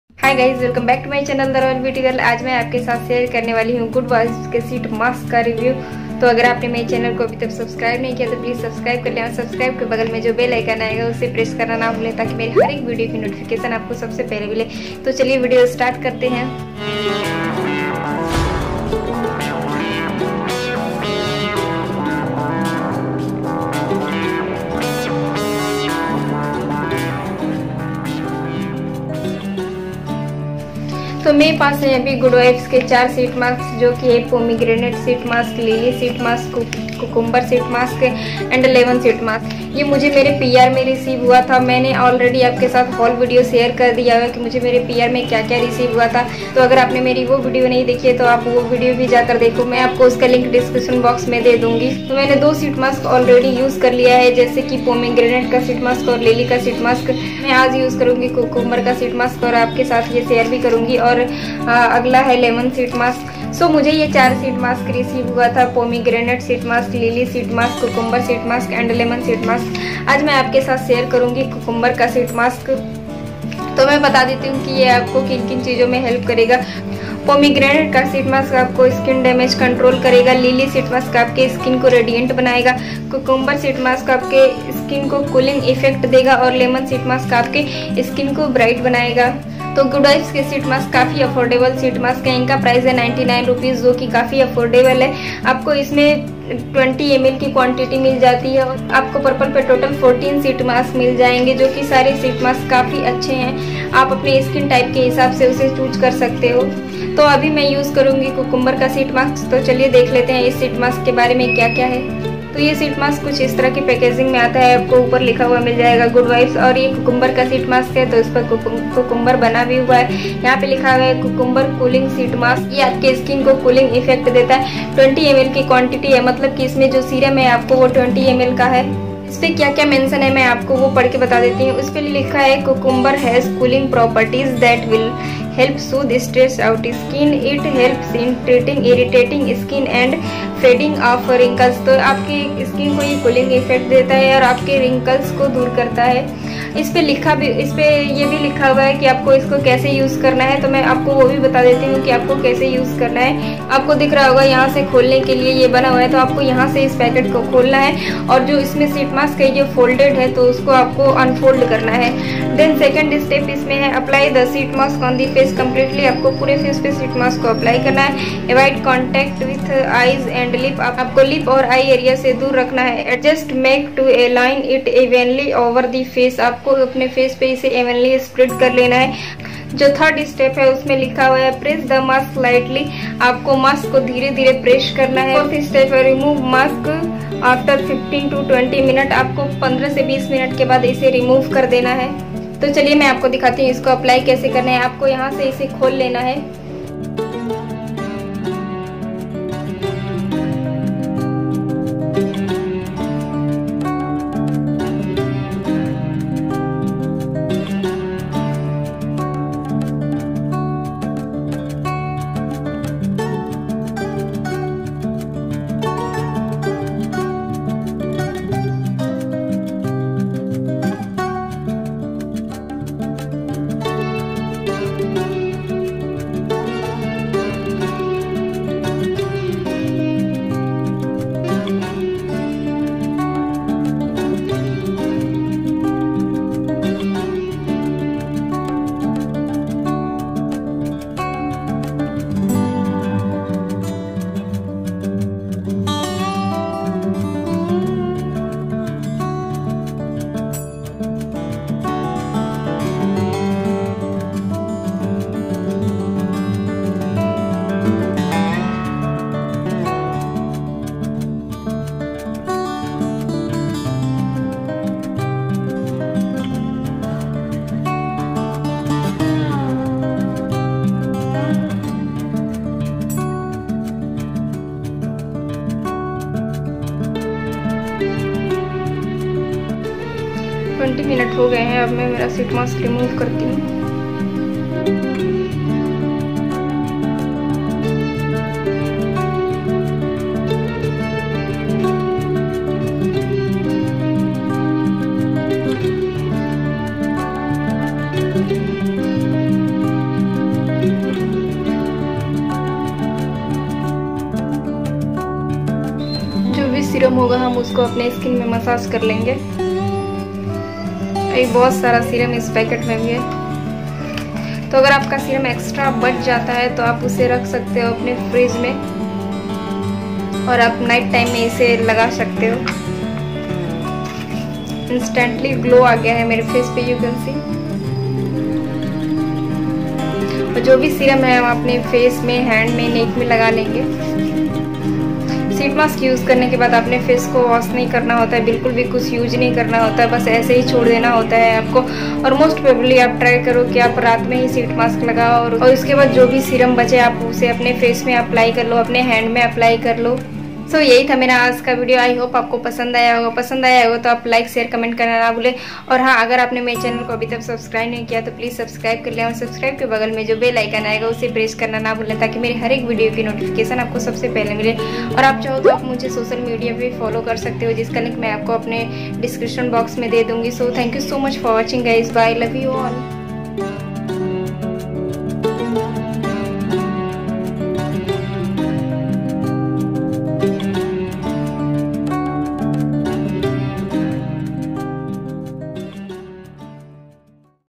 Good Vibes Sheet Masks Review With Demo ||The Royelbeauty Girl
Hey guys today I'm going to share with you a good vibe sheet mask review..  Hope you enjoy the video 😍😍

Good vibes sheet masks review with demo..

Copyright: In this video all the information given by the channel owner (Pushpanjali) so please don't use any information without permission..